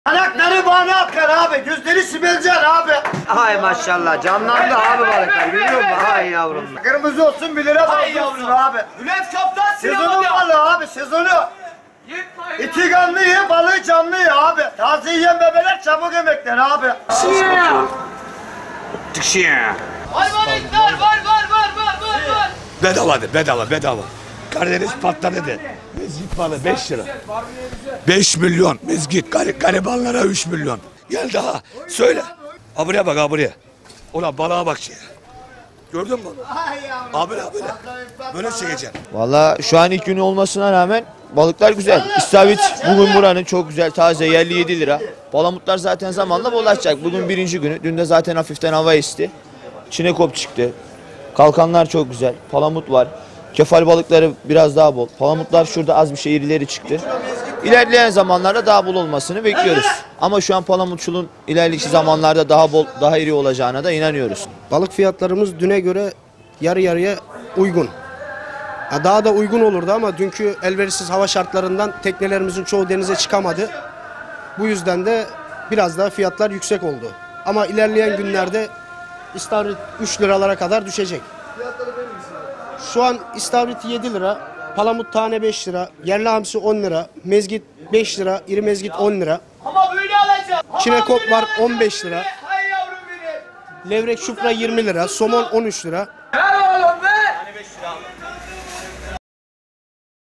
AV YASAĞI KALKTI BALIKÇILAR ÇILDIRDI! BALIKLARA 'BANU ALKAN' BENZETMESİ
Samsun’da 15 Nisan’da başlayan av yasağının kalkmasıyla gırgır tekneleri ve küçük teknelerle denize açılan balıkçılar "Vira Bismillah" dedi.Teknelerle gece açılarak ağlarını denize bırakan balıkçıların ağlarına en çok istavrit, çinakop, barbun, mezgit ve istavrit takıldı. Yasağın kalkmasıyla birlikte balıkçı tezgahları şenlendi. Yüzü gülen balıkçılar yaptıkları espirilerle müşterileri tezgahlara çekmeyi başardı